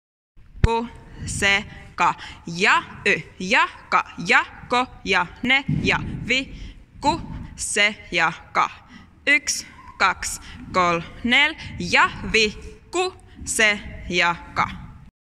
Jazz harjoitus 1
Lyhyt salin poikki tehtävä jazz - harjoite salin poikki.